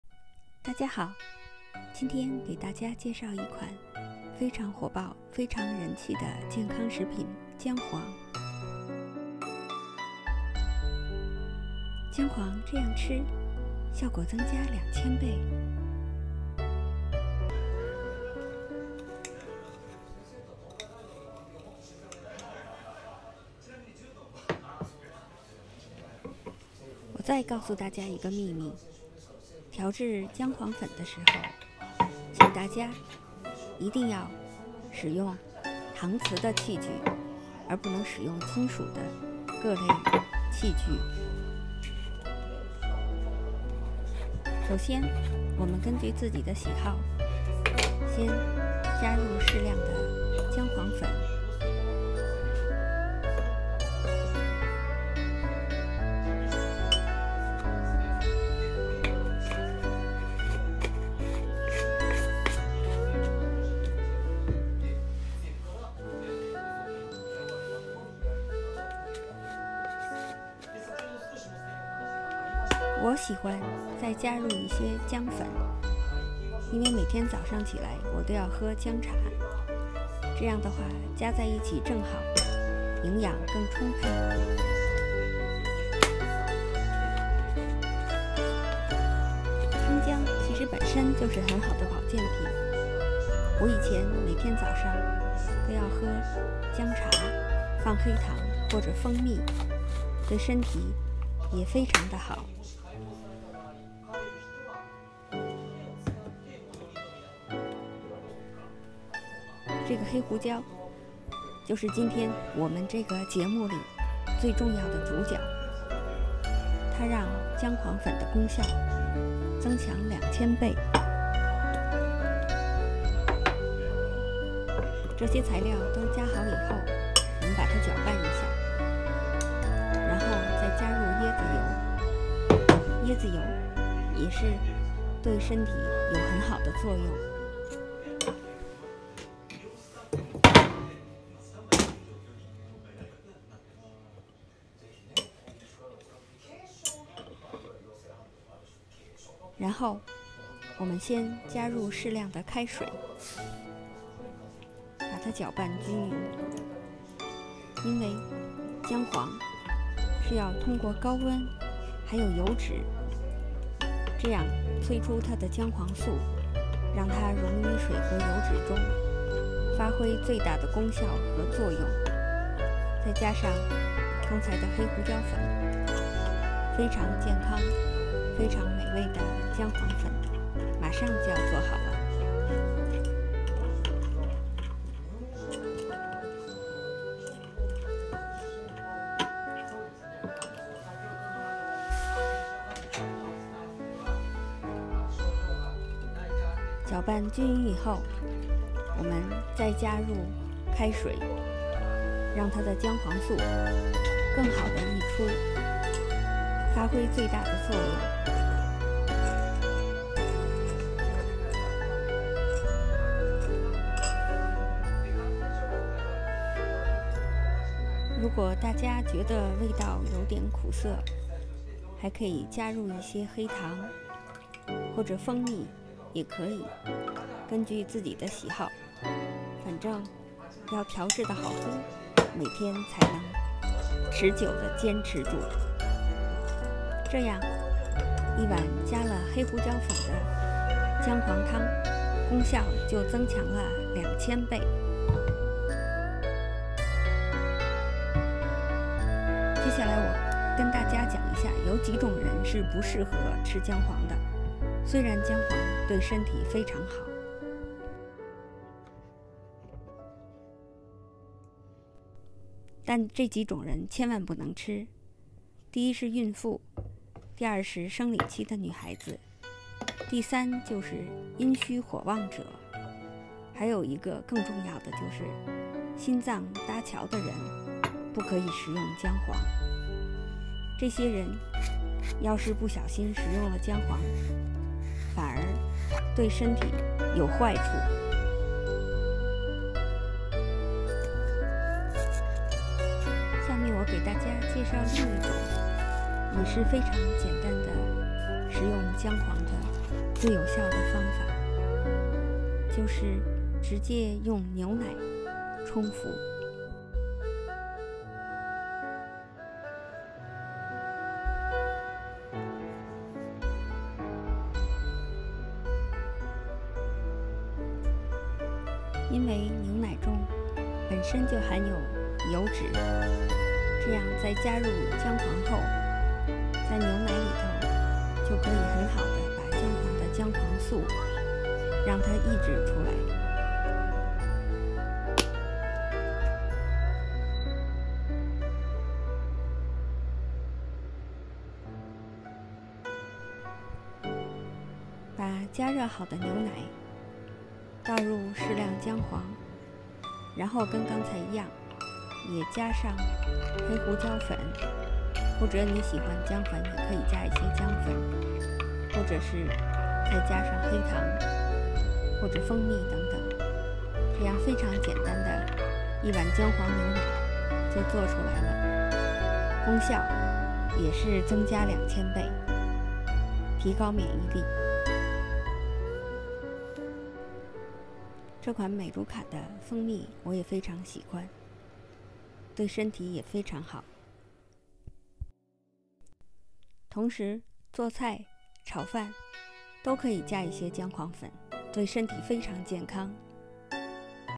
薑黃這樣吃 | 功效增强，提高免疫力 | 薑黃效果20倍！Turmeric is eaten like this, the effect is 20times!
薑黃這樣吃,功效增强，提高免疫力, 薑黃效果2000倍！Turmeric is eaten like this, the effect is 2000 times!